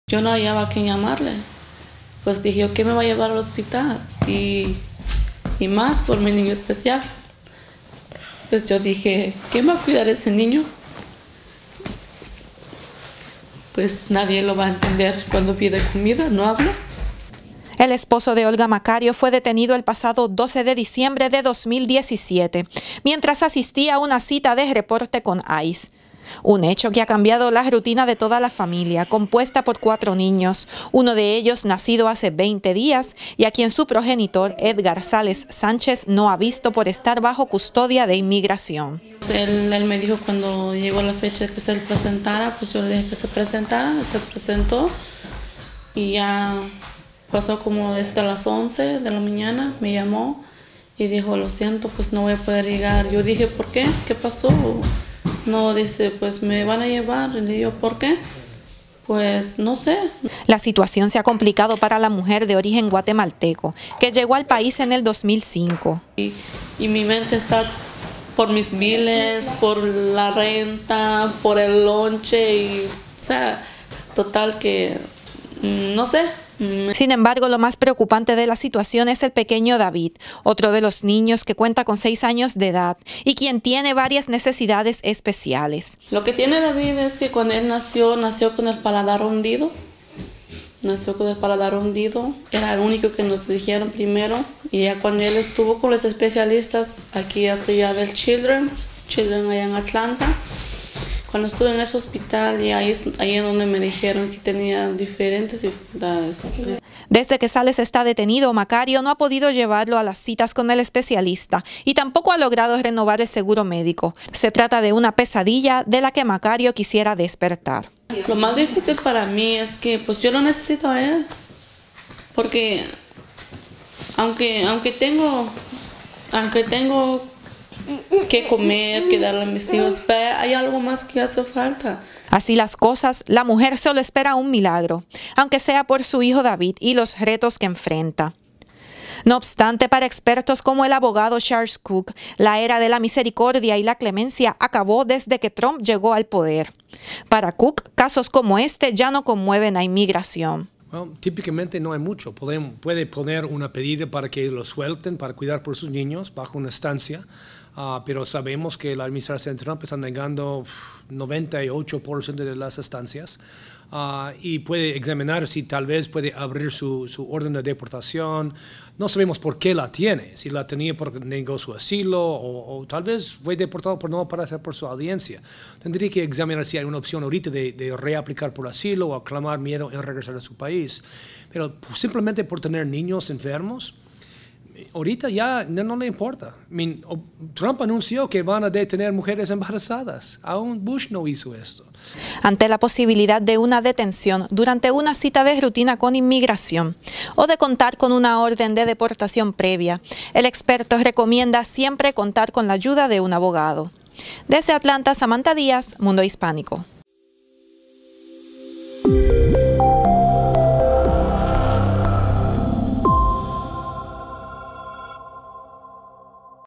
Detenido por ICE cuando asistió a una cita
No asombran las estadísticas. Y tampoco las acciones. Si hay una promesa de campaña que Donald Trump ha cumplido a cabalidad es la de su plan migratorio. El panorama ha cambiado significativamente en el último periodo para los indocumentados.